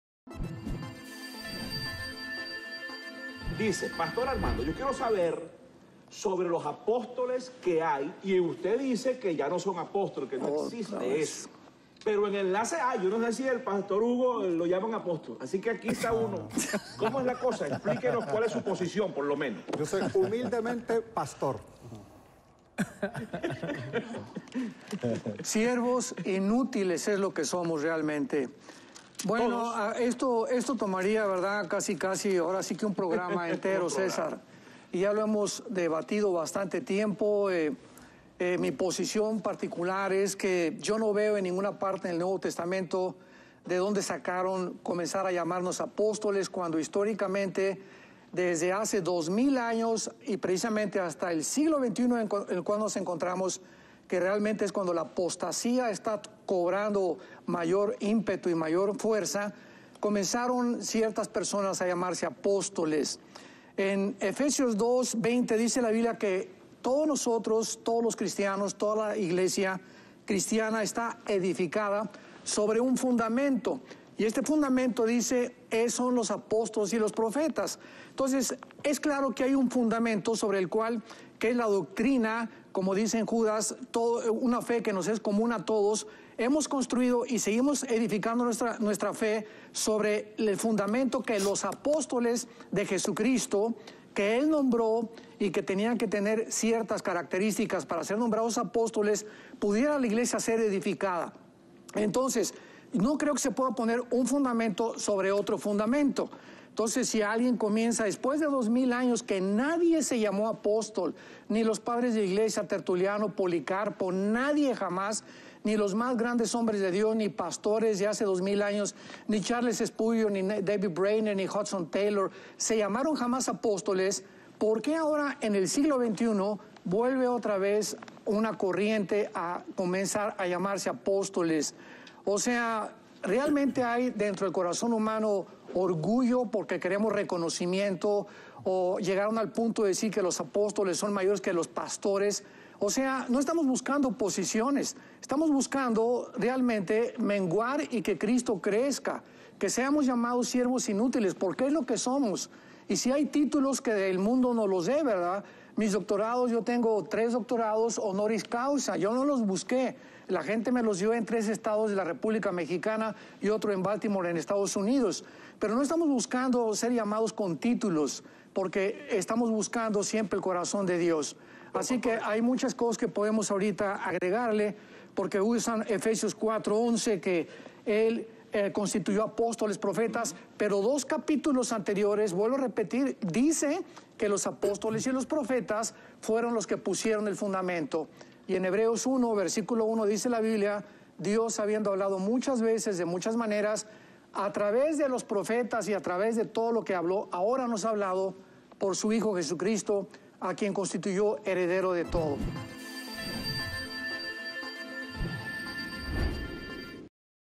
¿Existen apóstoles hoy en día? - Armando Alducin
El doctor Armando Alducin comparte su posición en cuanto a la figura del apostolado en estos tiempos.